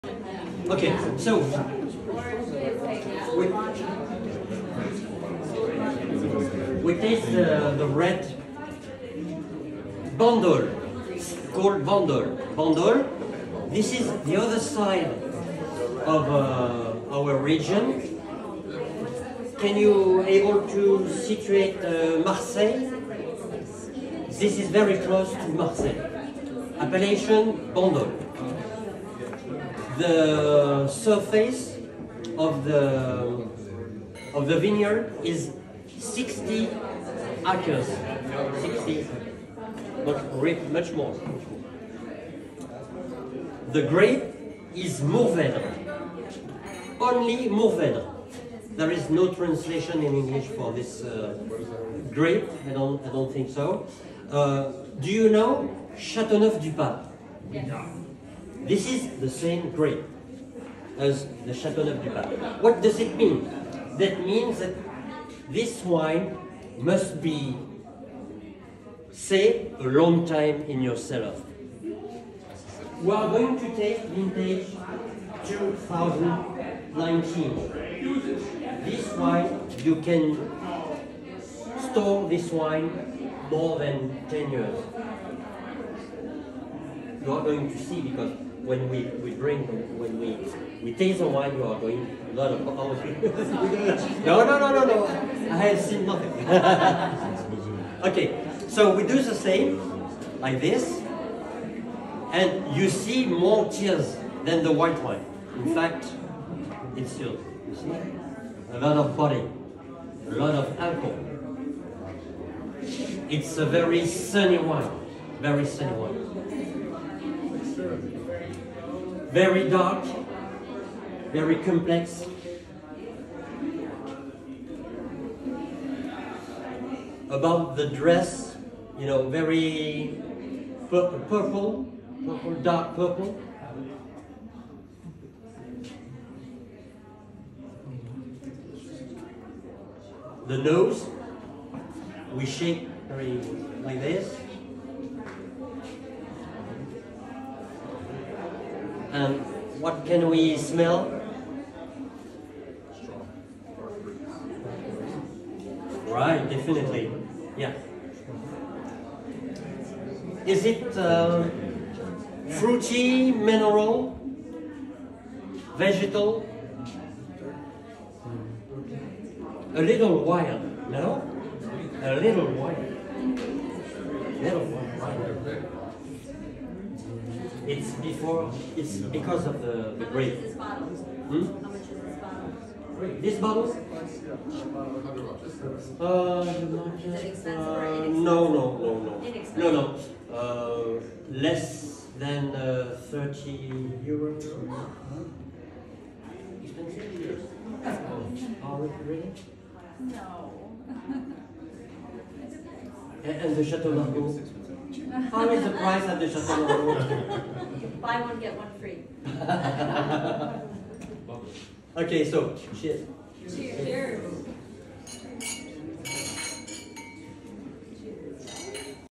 The Hôtel de Paris Monte-Carlo Wine Cellars Sommelier Monaco Red Wine Instruction! Luxury Trip Girl
Arranged by American Express - a tour of the The Hôtel de Paris Monte-Carlo Wine Cellars with a presentation by a Sommelier in Monaco on the proper drinking of red wine! Luxury Trip Girl